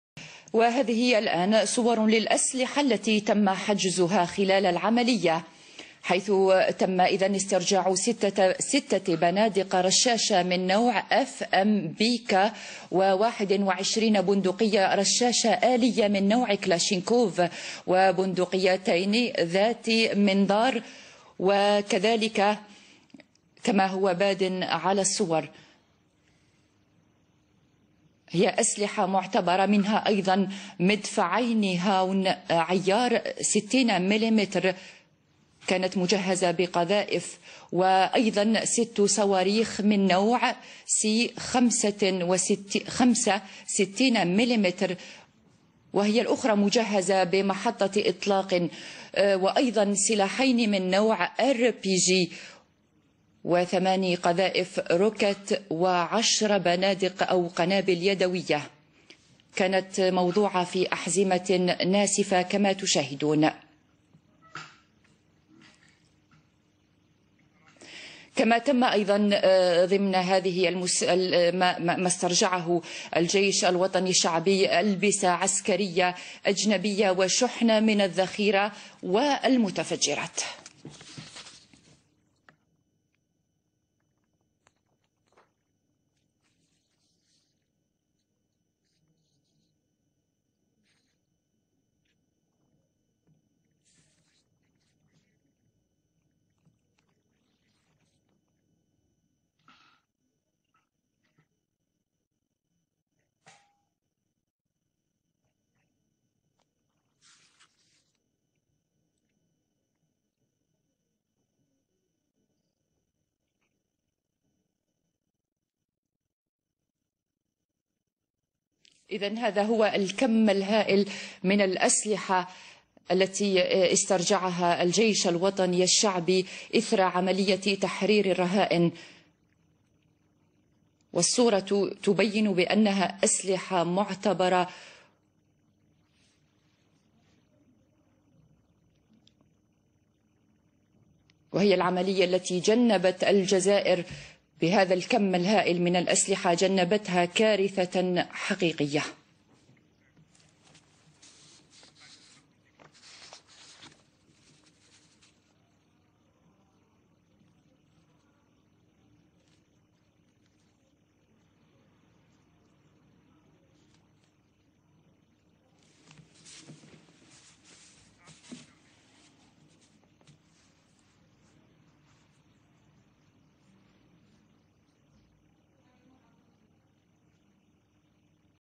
Algérie In Amenas : la prise d'otages se termine dans le sang et récupération d'un arsenal
L'armée a récupéré un arsenal important : "six fusils-mitrailleurs, 21 fusils, deux fusils à lunettes, deux mortiers 60 mm avec roquettes, 6 missiles avec rampes de lancement, deux RPG7 avec huit roquettes, 10 grenades disposées en ceintures explosives", selon le communiqué. Des "tenues militaires étrangères et un stock de munitions et d'explosifs" ont été trouvés.
In Amenas : la prise d'otages se termine dans le sang
طالبت حكومات غربية السلطات الجزائرية بتفسير طريقة تعاملها مع حادث احتجاز رهائن في منشأة الغاز بعين أميناس جنوب شرق الجزائر من قبل مسلحين مرتبطين بتنظيم القاعدة، وذلك بعد الإعلان عن انتهاء عملية عسكرية لتحرير الرهائن أدت لمقتل 23 رهينة و32 من المسلحين، وفقدان أثر عدد آخر من العاملين الأجانب في المنشأة الواقعة في الصحراء.
Vingt-trois otages étrangers sont morts durant les quatre jours qu'a duré l'attaque du site gazier d'In Amenas. Trente-deux terroristes ont, eux, été abattus par les forces spéciales algériennes, selon un bilan provisoire.
    Vingt-trois captifs ont péri durant les quatre jours de prise d'otages sur un site gazier en Algérie, alors que 32 ravisseurs ont été tués par les forces spéciales de l'armée qui ont mené un assaut contre le complexe, selon un bilan provisoire officiel.
    Les forces algériennes ont pu libérer "685 employés algériens et 107 étrangers" et ont abattu "32 terroristes", a indiqué le ministère de l'Intérieur dans un communiqué. Vingt-trois otages ont péri, a-t-il ajouté sans préciser leurs nationalités. Parmi les 32 ravisseurs tués figurent trois Algériens. Les autres étaient de différentes nationalités non précisées et comprenaient des experts en explosifs, a-t-il précisé.
    Dix Japonais étaient toujours portés manquants plusieurs heures après la fin de la prise d'otages sur le site gazier du sud-est algérien, a déclaré dimanche matin un porte-parole de la compagnie JGC Corp qui employait 78 personnes sur le site. Dans la nuit de samedi à dimanche, le Premier ministre japonais Shinzo Abe a déclaré avoir reçu des autorités algériennes "une information grave" sur la situation des otages japonais.
Le Premier ministre japonais Shinzo Abe

    Témoignage d'un des chefs de la sécurité du complexe gazier d'In Amenas
    No Flash warning
    Pour profiter pleinement de toutes les fonctionnalités de FRANCE24.COM, cliquez ici pour télécharger la dernière version de Flash Player.

    Le chef présumé des ravisseurs sur un site gazier en Algérie avait menacé de "faire exploser" les otages si l'armée algérienne s'approchait trop près de l'usine, dans un enregistrement diffusé samedi par l'agence de presse mauritanienne ANI.

    Alors que des pays occidentaux se sont inquiétés de l'opération algérienne, le président français François Hollande a estimé que l'Algérie avait eu "les réponses" les "plus adaptées" car "il ne pouvait y avoir de négociation" avec les ravisseurs.

Déclarations de François Hollande
No Flash warning
Par FRANCE 24

    Le groupe "Signataires par le sang" de l'Algérien Moktar Belmokhtar avait pris mercredi des centaines d'Algériens et des dizaines d'étrangers en otage sur le site gazier, en disant que cette opération était menée notamment en représailles à l'intervention militaire française au Mali qui bénéficie du soutien d'Alger.

    D'après des "sources djihadistes" citées par l'agence mauritanienne ANI, le
    Syndiquer le contenuPORTRAIT
        Mokhtar Bel Mokhtar, un professionnel du djihad et des prises d'otage
        ALGÉRIE